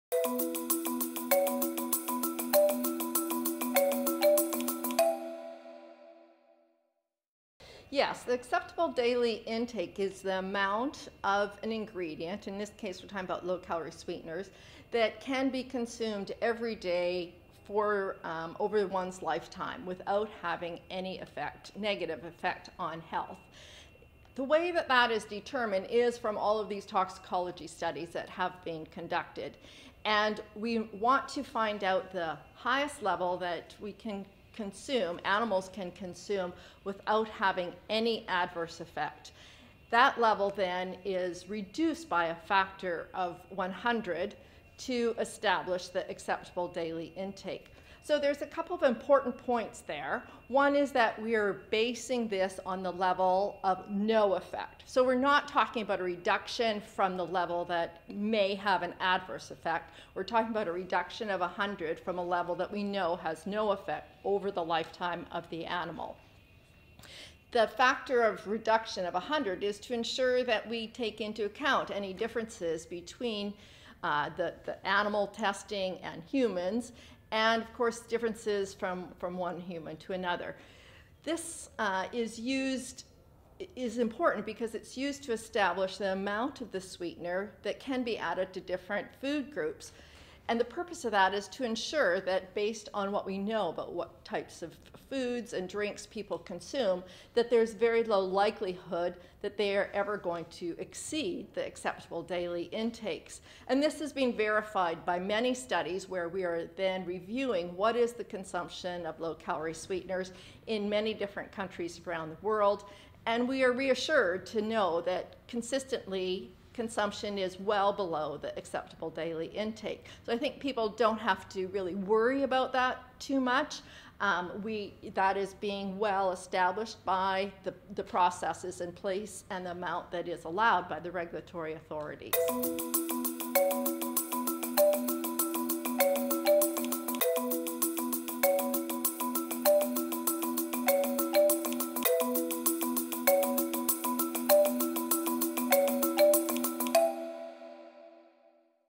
What is the Acceptable Daily Intake (ADI)?
On the occasion of the 3rd ISA Conference in November 2018 in London, the ISA interviewed Dr Berna Magnuson, Health Consultant in Mississauga, Canada, about low calorie sweeteners' safety and acceptable daily intake (ADI).